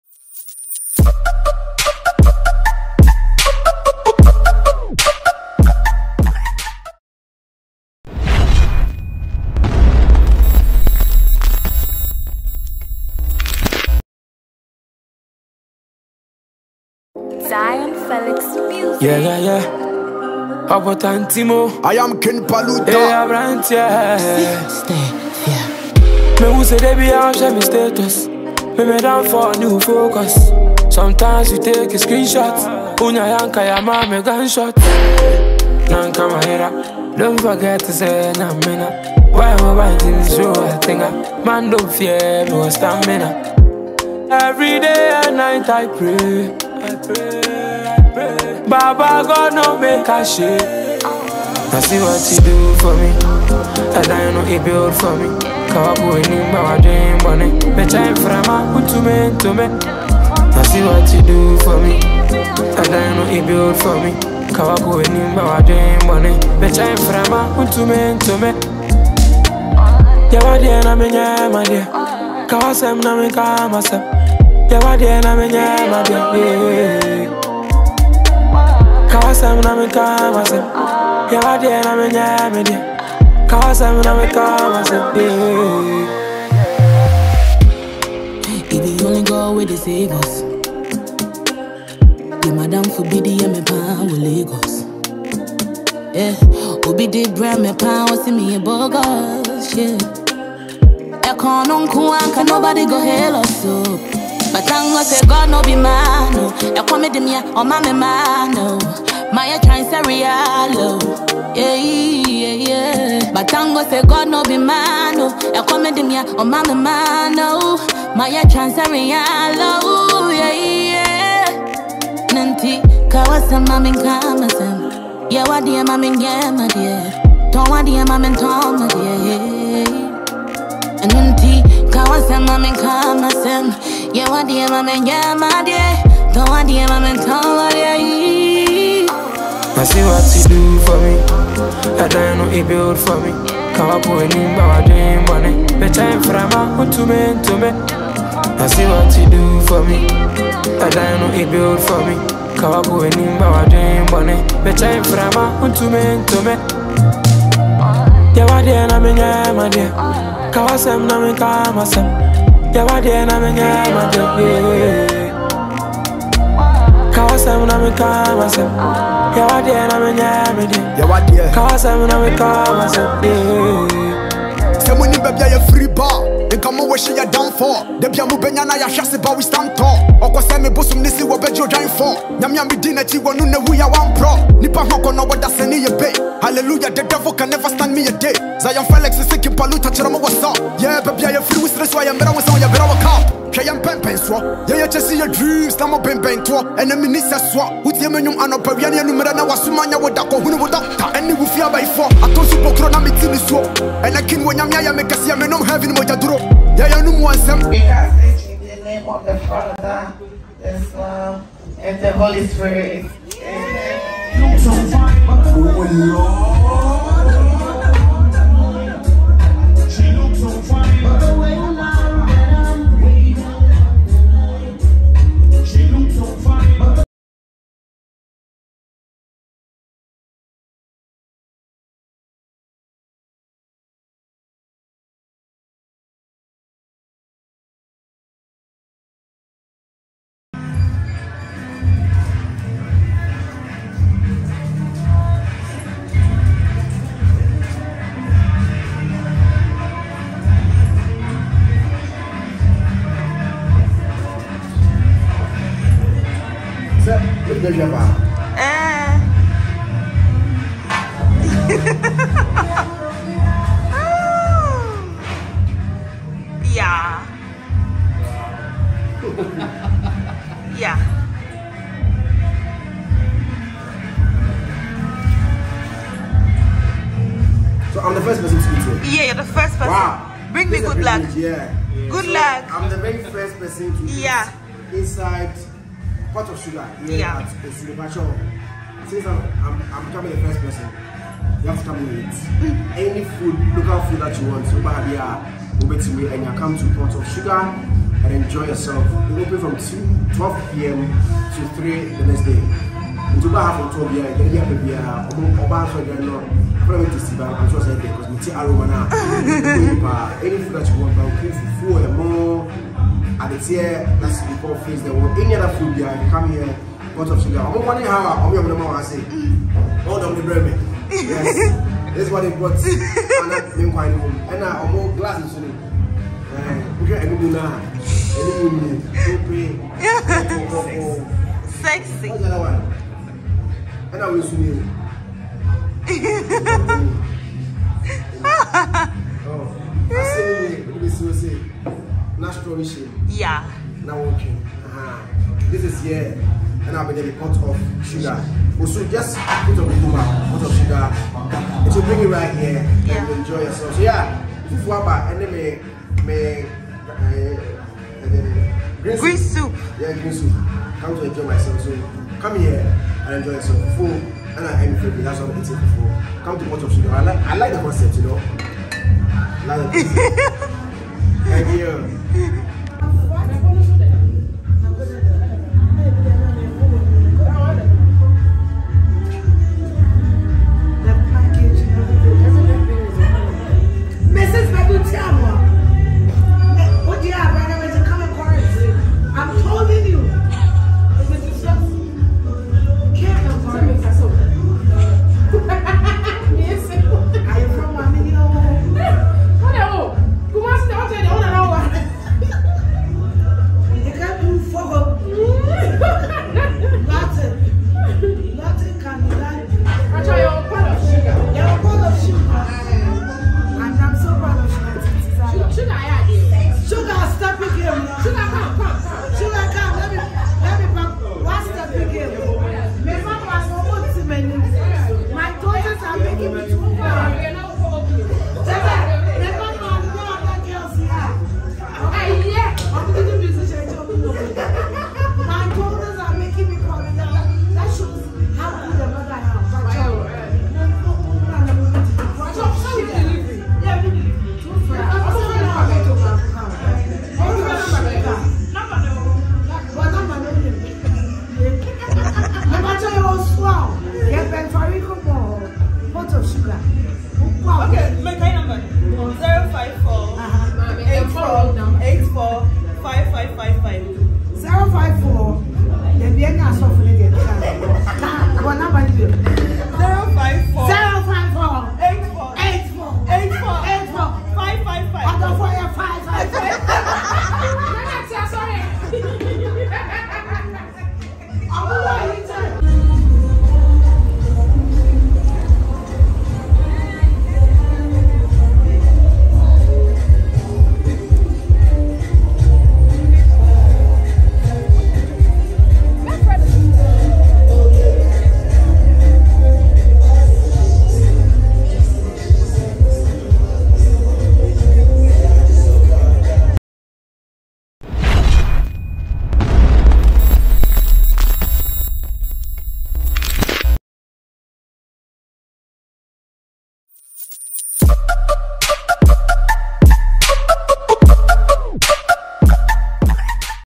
Shugatiti Opens Restaurant, A P3nis-Like Pot At The Restaurant Causes Stir Online
Shugatiti got everyone talking about her new restaurant